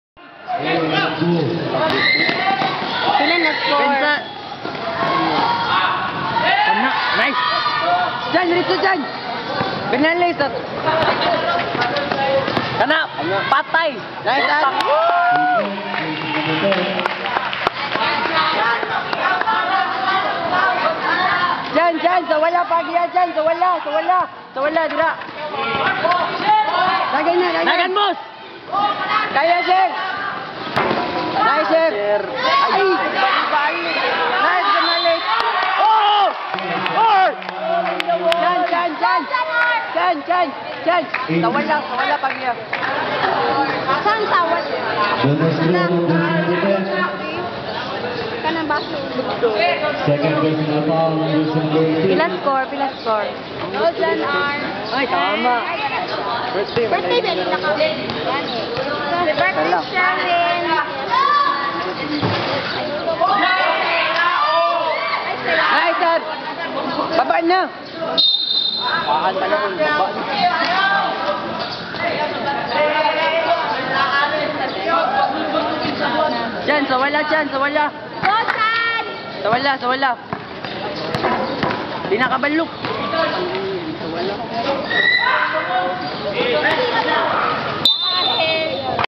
ASSUMPTA BASKETBALL GAME
championship game 2.
assumpta school vs liceo.
1st quarter

winner : AST ! :]